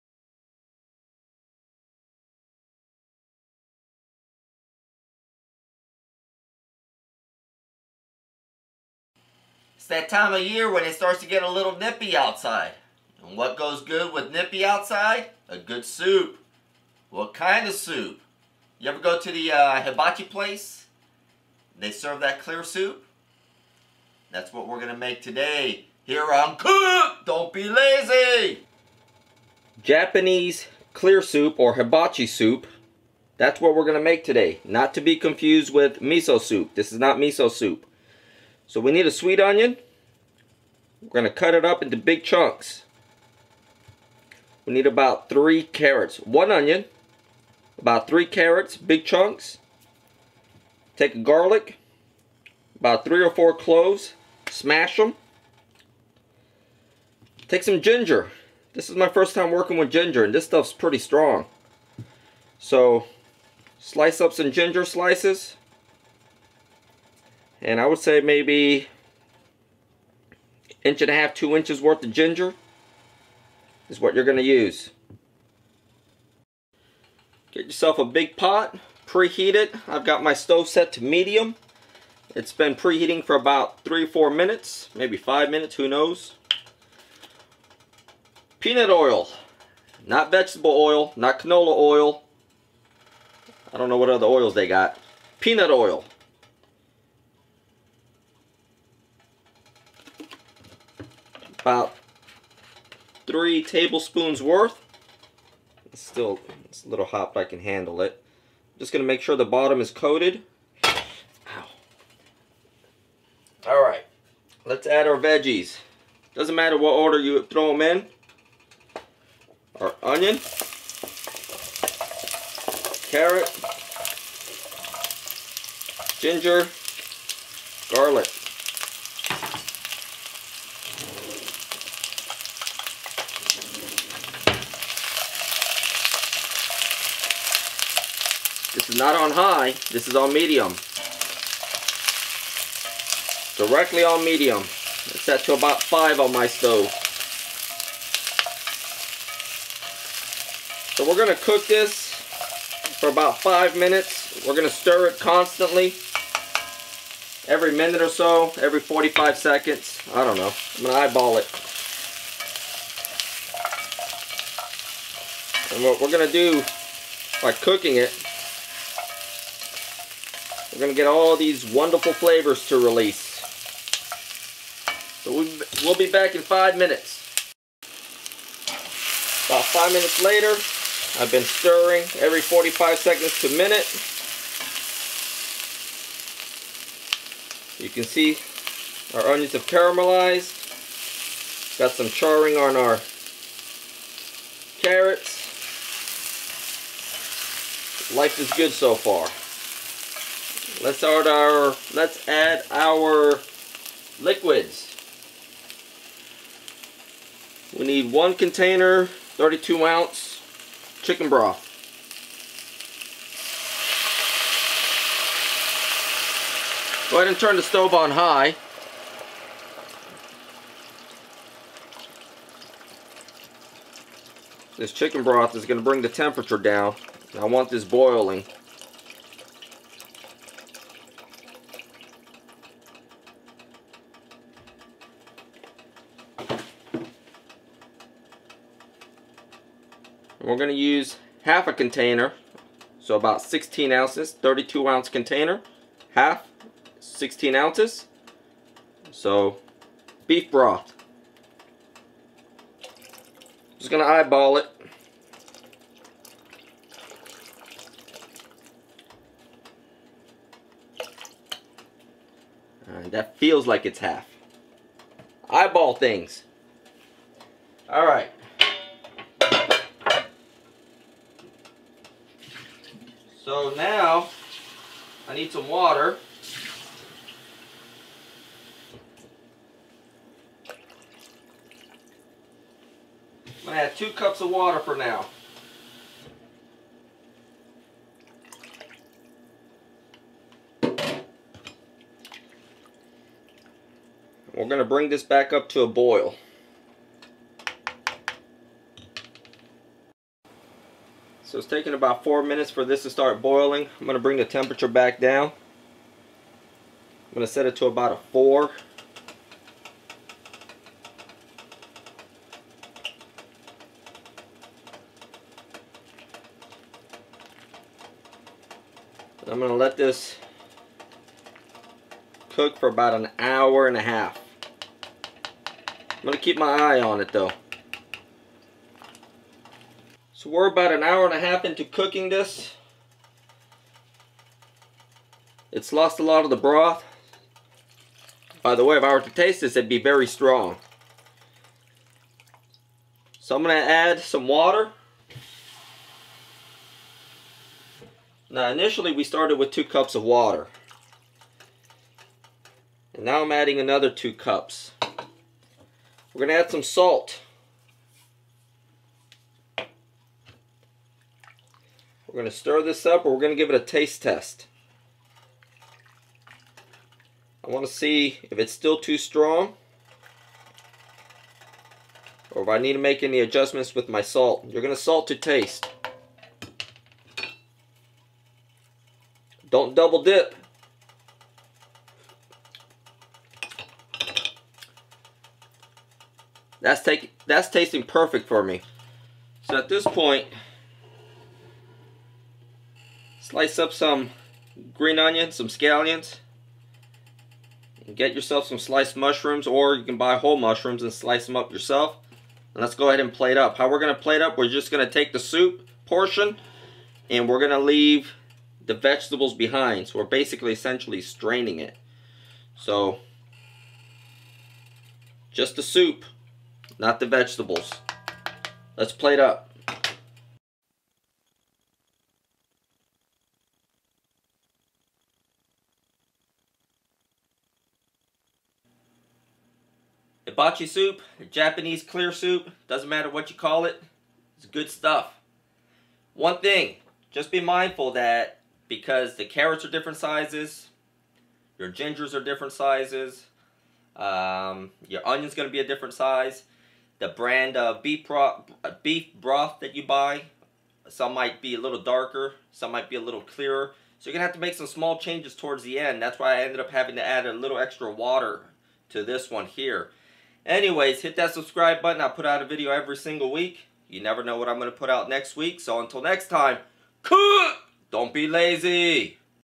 Hibachi / Japanese Clear Soup | COOK - Don't Be Lazy
Japanese Clear Soup / Hibachi Clear Soup - everyone's favorite at the Hibachi restaurant! Now you can have it at home!

Ingredients:

Sweet Onion
Carrots
Ginger
Garlic Cloves
Peanut Oil
Chicken Broth
Beef Broth
Salt
Green Onion / Scallions
Mushrooms